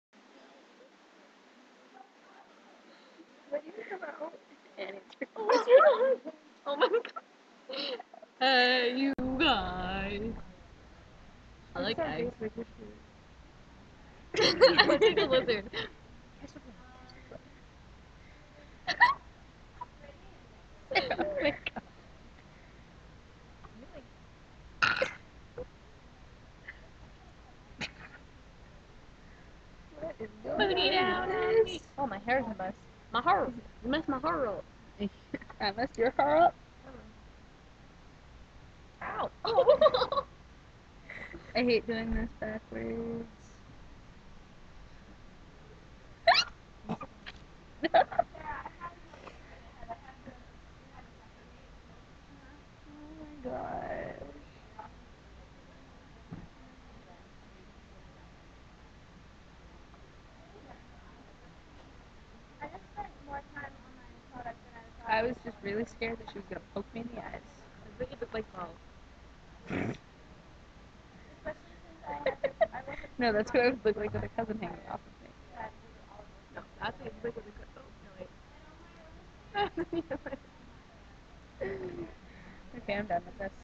cousinly love
me and my two cousins. they both thought I was taking a picture. this was taken during one of our idependent periods in our last year of high school so we got a little bored as you can tell